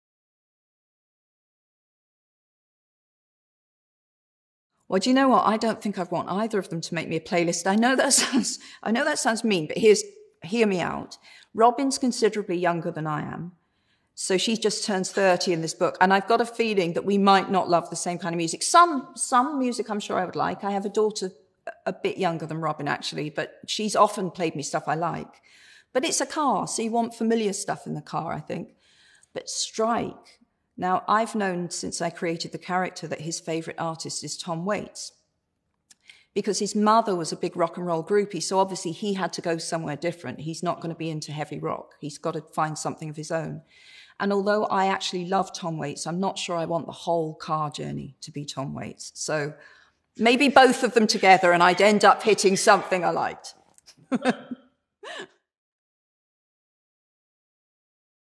Robert Galbraith (J.K. Rowling) on who should choose the music on a road trip, Strike or Robin?
Robert Galbraith (J.K. Rowling) answers: If you were going on a road trip, who would you prefer to create the playlist, Strike or Robin?

Published on November 18th, 2020, on the official Robert Galbraith Instagram account.

No commercial purposes. Only to keep an archive of J.K. Rowling's appearances.